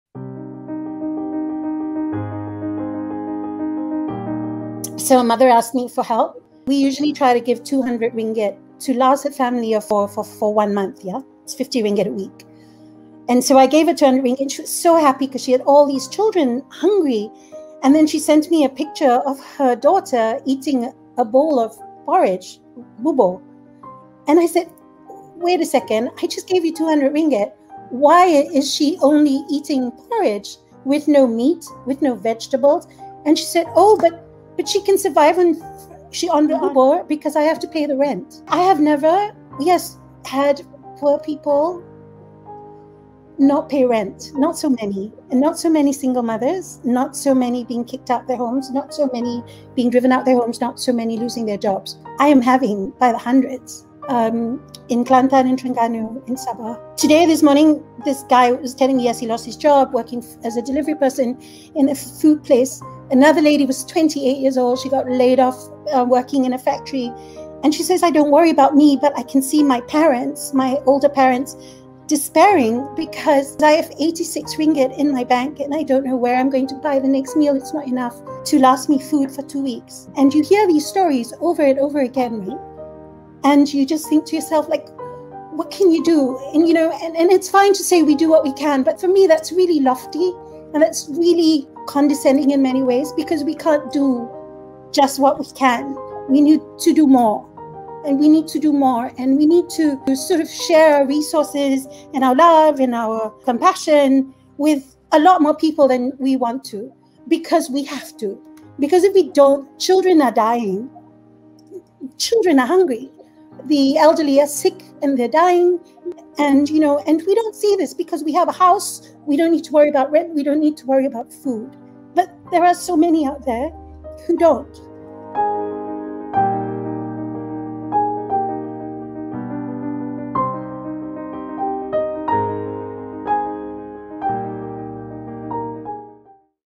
We Need To Share Our Resources, Our Love, And Our Compassion. Seeing The Unseen of Poverty (Part 2)
In the next episode of “Seeing the unseen of poverty”, we have Tini Zainudin, Co-Founder of Yayasan Chow Kit sharing her experiences and thoughts on what we can do to help the needy during struggling times like today.